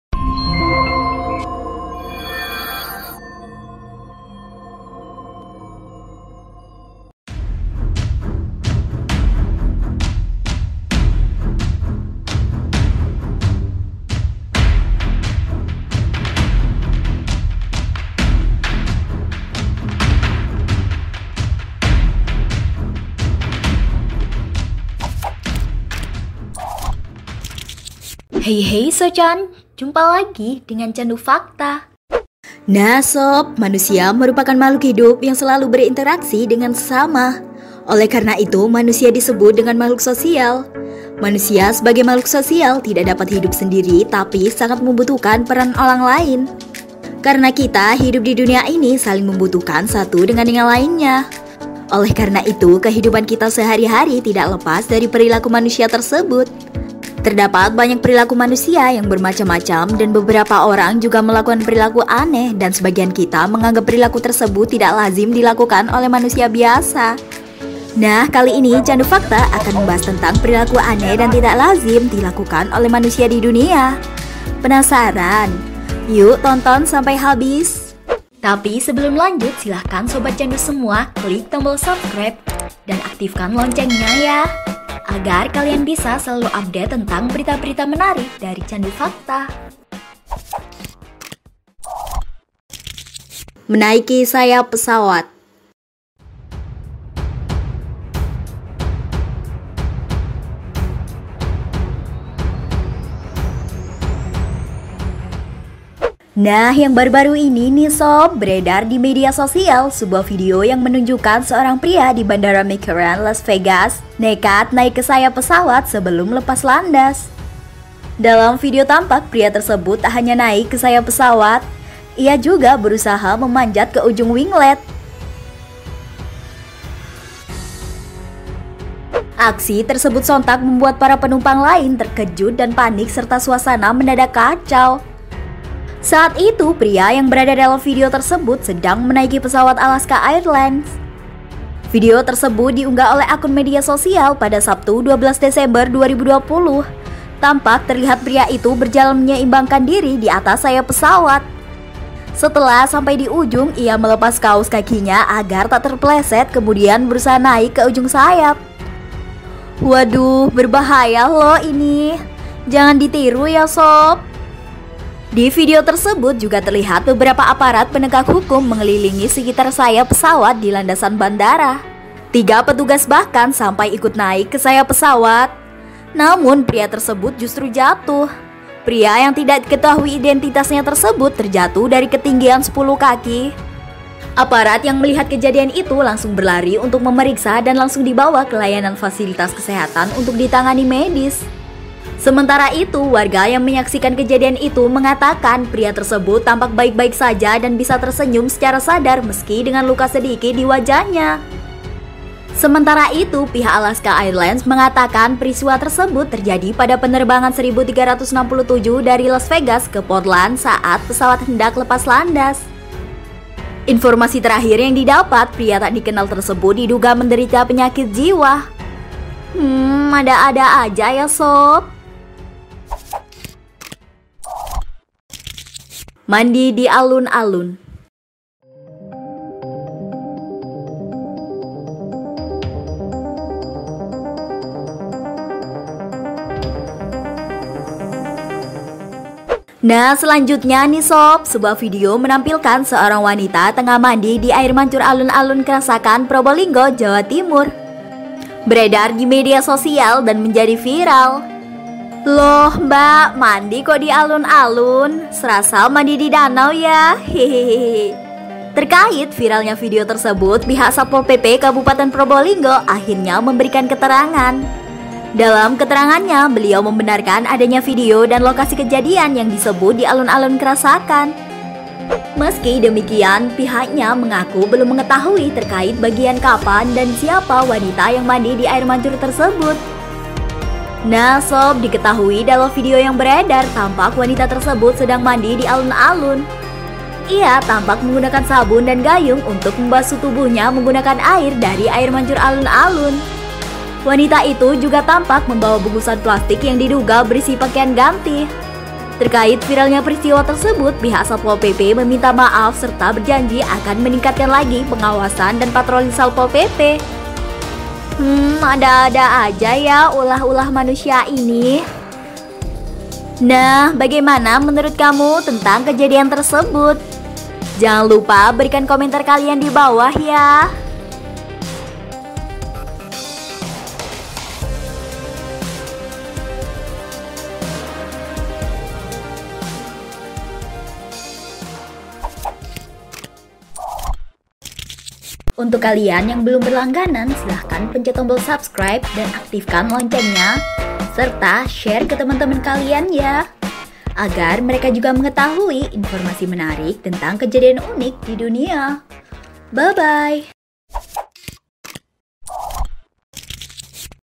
Viral !! Pria Nekat Naik Sayap Pesawat Saat Lepas Landas. Dan Akhirnya...
Dalam video tampak pria tersebut tak hanya naik ke sayap pesawat, ia juga berusaha memanjat ke ujung winglet. Aksi tersebut sontak membuat para penumpang lain terkejut dan panik serta suasana mendadak kacau.